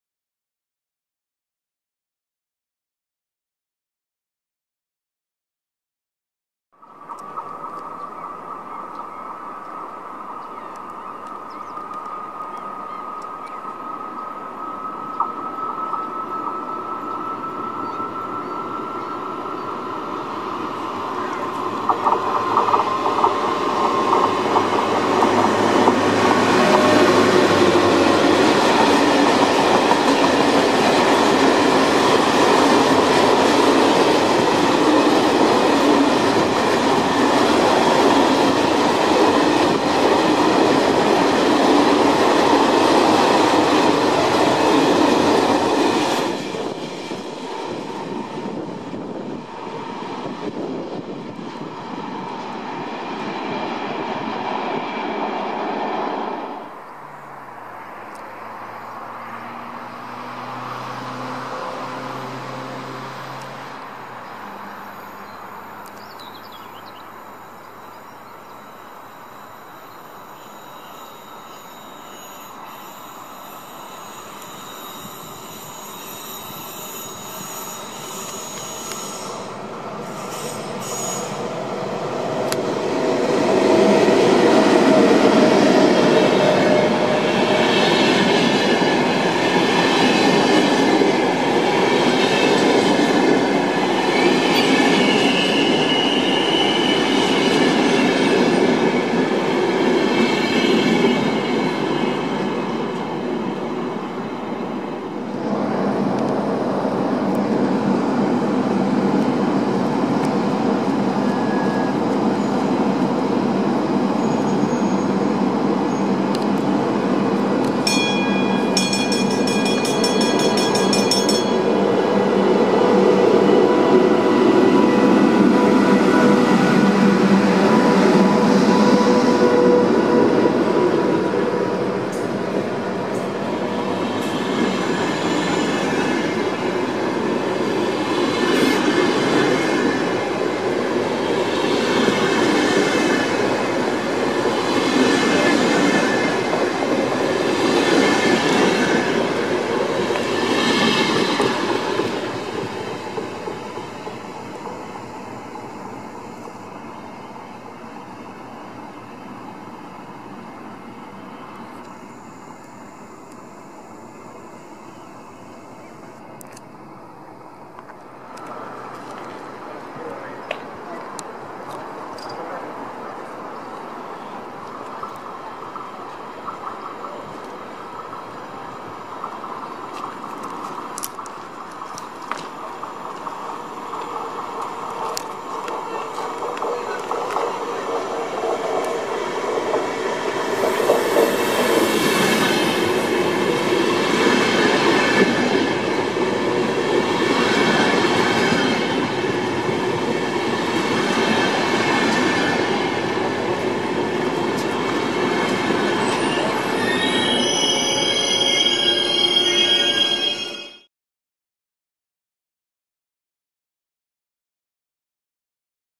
Kuvailua Imatralla - Trainspotting at Imatra 12.7.2023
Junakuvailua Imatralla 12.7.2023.
Junat:
T 55848
IC 111
SAA 62111
IC 12